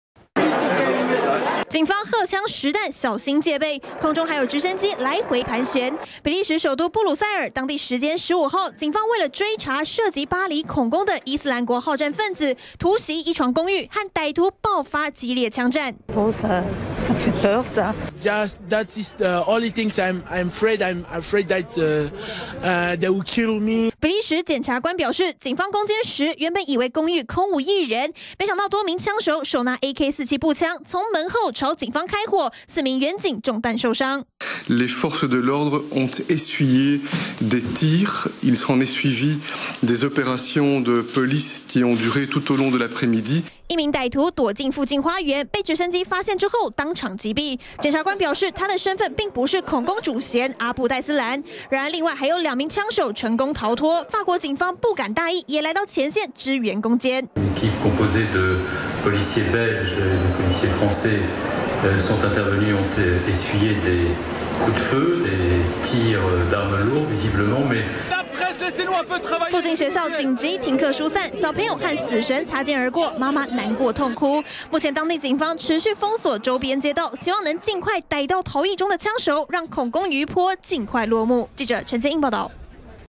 20160316中天新聞 追捕巴黎恐攻嫌犯 比國槍戰擊斃1槍手
巴黎恐攻事件餘波盪漾！比利時警方在當地時間15號，為了追查涉案的伊斯蘭國份子，攻堅突襲一棟公寓，和藏匿在內的歹徒激烈駁火。一名歹徒遭擊斃，另外還有2人在逃。目前比利時和法國警方聯手，封鎖周遭街道，追查槍手下落。